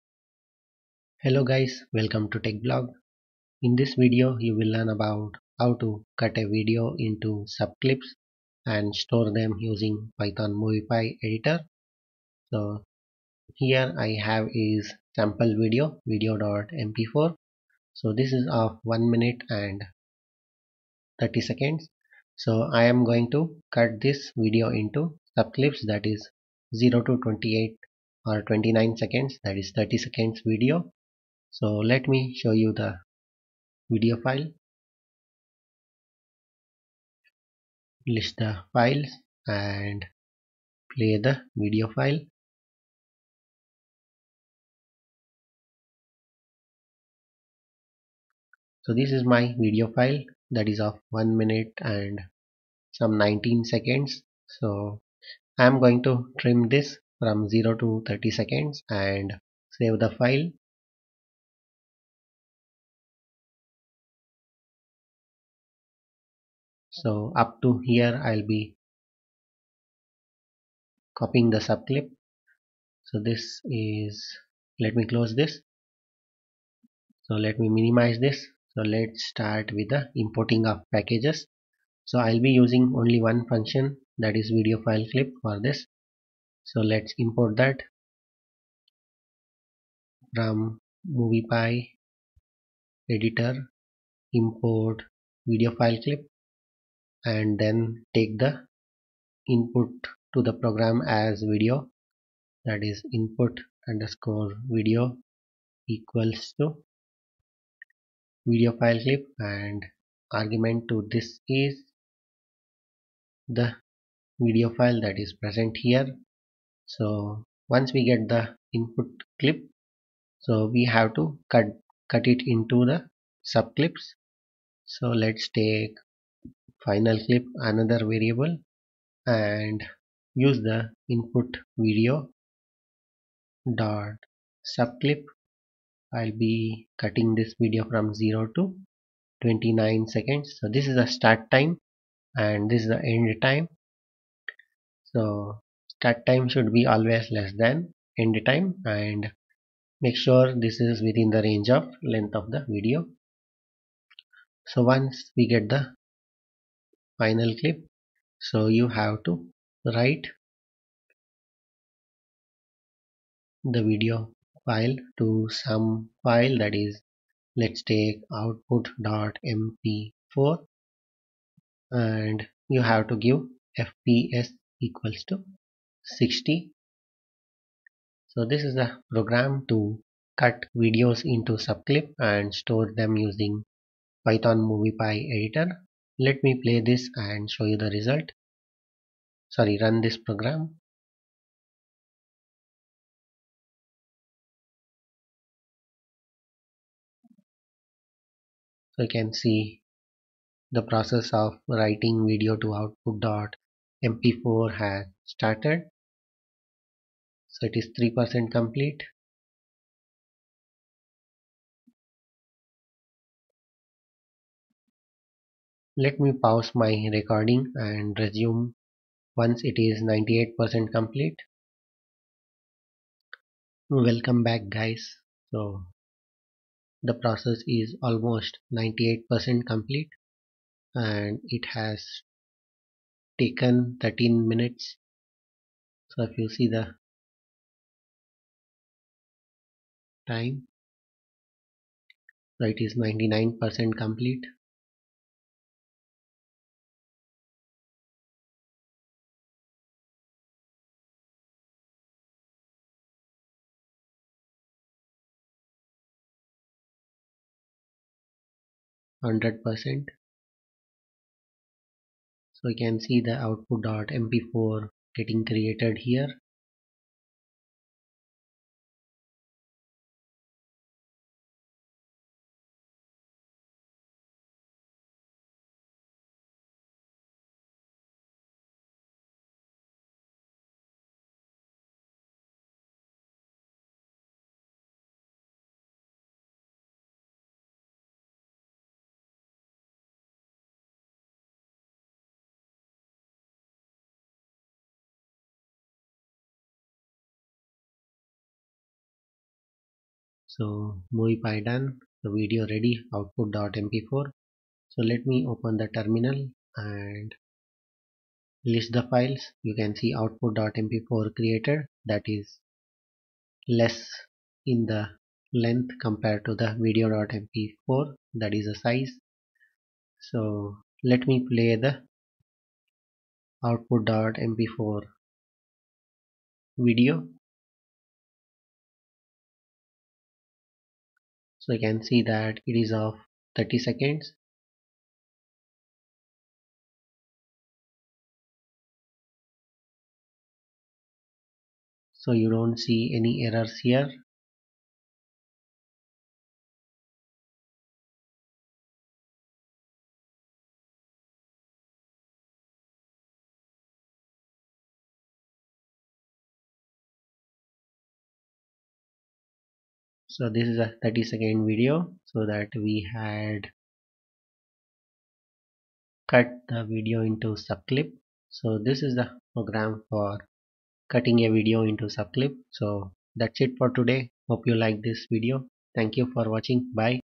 Cut Video into Sub Clip and Store Them Using Python Moviepy || Extract Sub Clip from Video File Clip
Hey Guys in this video I tried to cut or extract sub clip from lengthy video file

Steps Used
a) Import required packages
b) Get video into program
c) Extract sub clip
d) Write the subclip